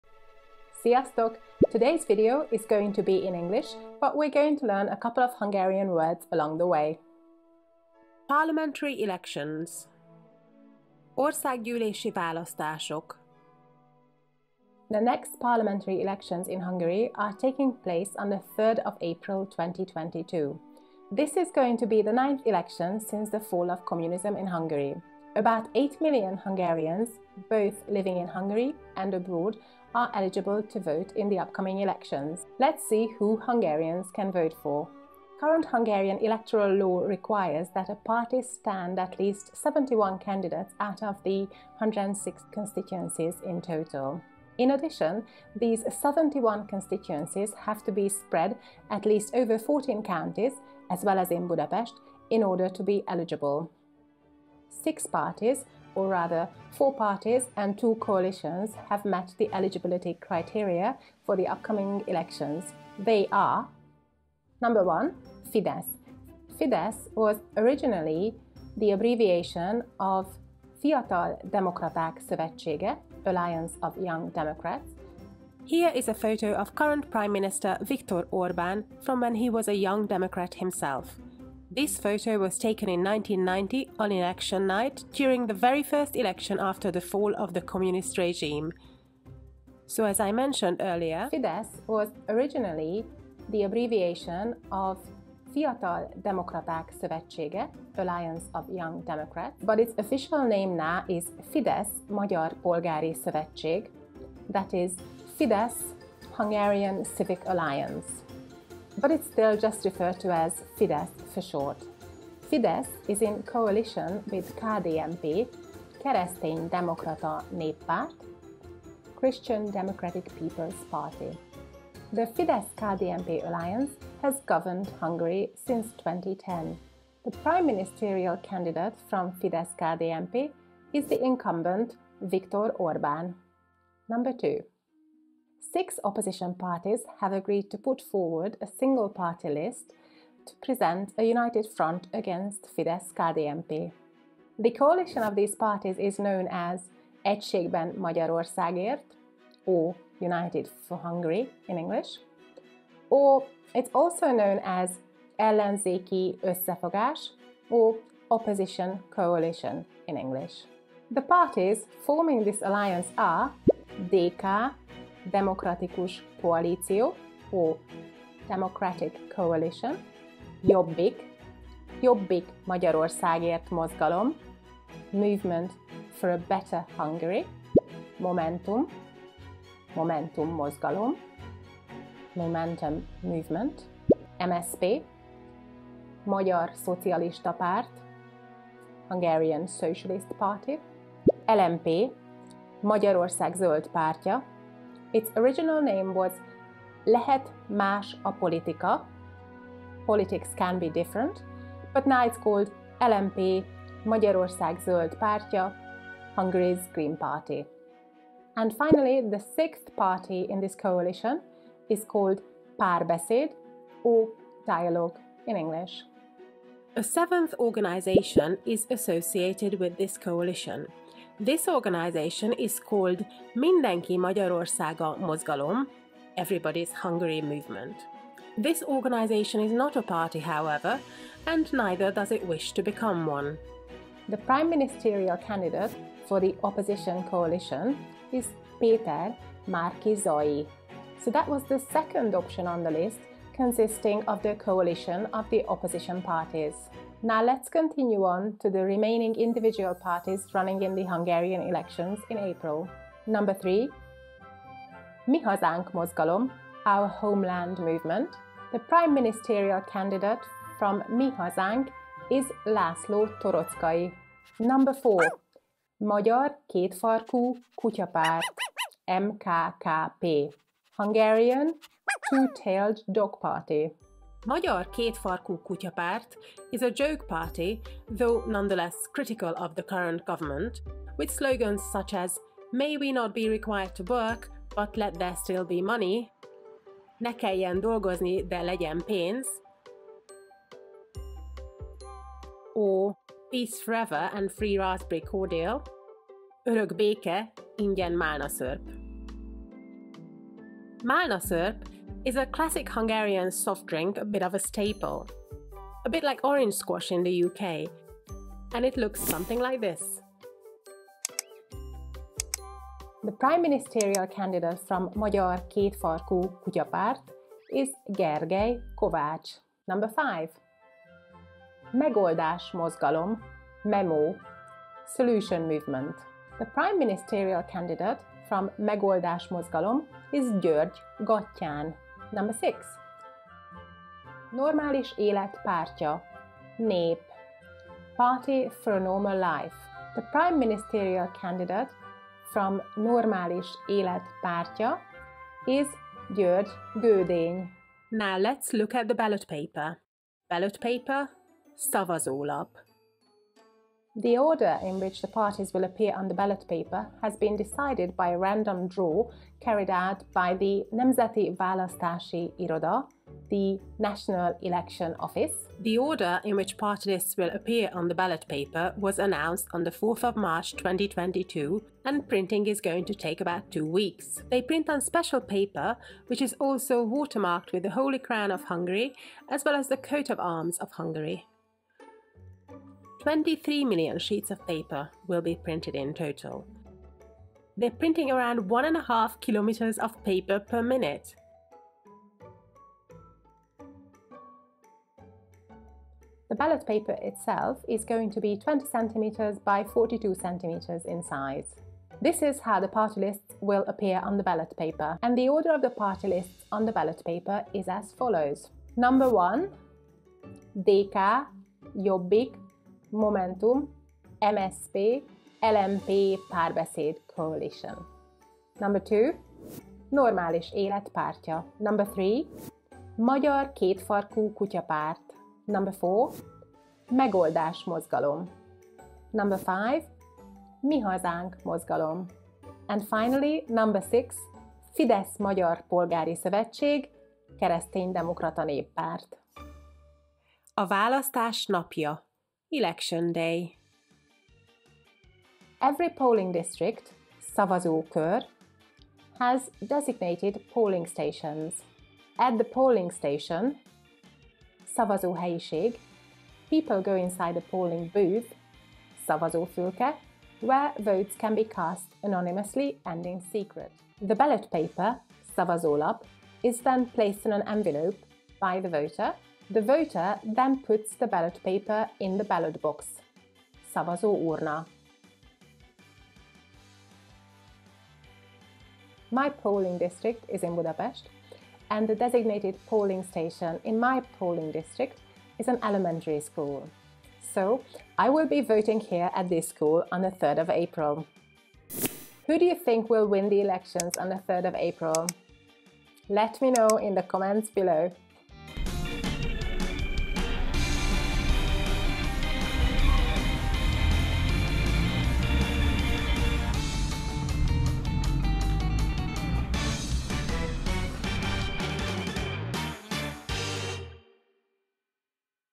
HUNGARY ELECTIONS 2022 - Parties and candidates
0:00 - Intro - Hungary Elections 2022 (Magyar választások)
00:32 - 05:50 -  The parties (A pártok)
05:50 - 02:26  - The ballot paper (A szavazólap)
07:20 - 08:36 -  The day of the election (A választás napja)

The Hungarian Elections (or Magyar választások - in Hungarian) are coming up on 03 April 2022. In this video, you can find out the names of the parties Hungarians can vote for as well as the names of the prime ministerial candidates. I also show you what the ballot paper will look like and in what order parties feature on it. Finally, we have a look at what Hungarians do on election day and I even show you my own polling station. As always, you can learn a couple of Hungarian words along the way.